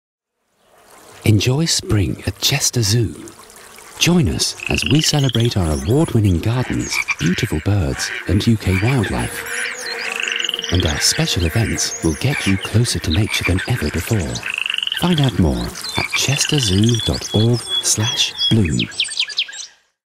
Bloom at Chester Zoo: A celebration of gardens, birds and UK wildlife.
Nature comes alive in spring and the zoo can be a relaxing and inspiring place to be. This year we are giving you the opportunity to get closer to nature than ever.

Watch the fluttering of the butterflies, listen to the song of our exotic birds in our awe-inspiring free flight aviaries and notice the buds emerging in our award winning gardens.

We'll take you behind the scenes, introduce you to the experts who create our unique and beautiful landscapes and you'll have the opportunity to become a UK wildlife conservationist too.

With a season of special events and the opening of our brand new wildlife reserve, escape to Chester Zoo this spring.